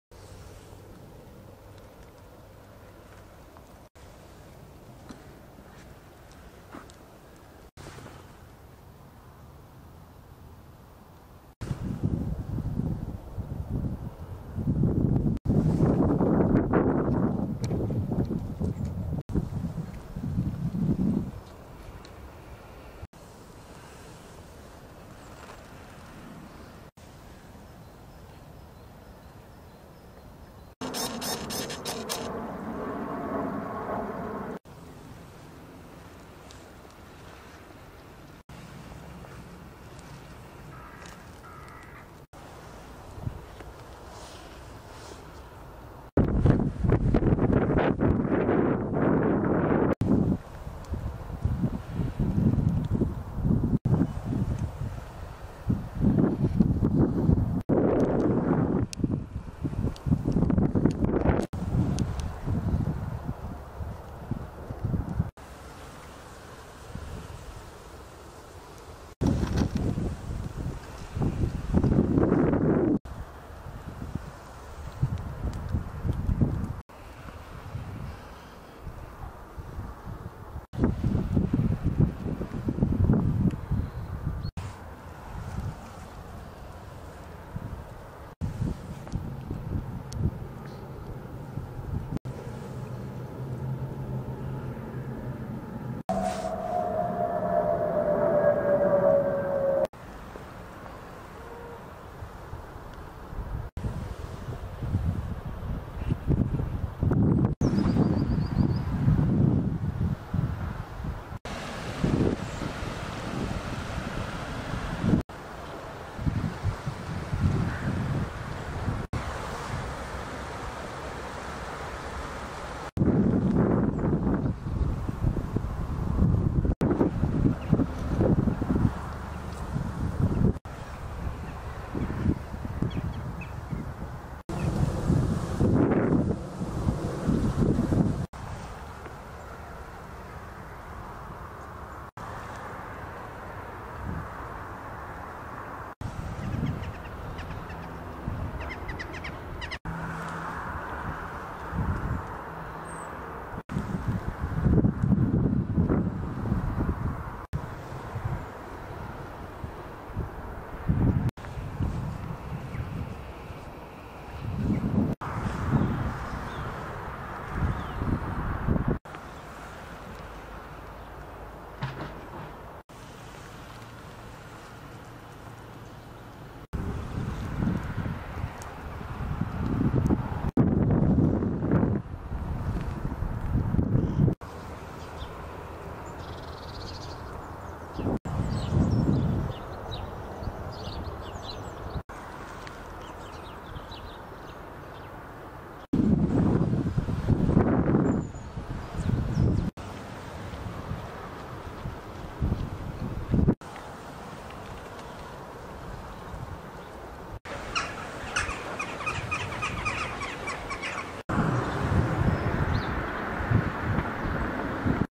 Canal Walk Bridge 177 to the Big Lock Middlewich
Walking towards the "Big Lock" Pub In Middlewich Cheshire from bridge 177 on the trent and mersey canal With An EOS M & a tokina 11-16 lens

Stopping at all the turns in the canal for a 4 second pan shot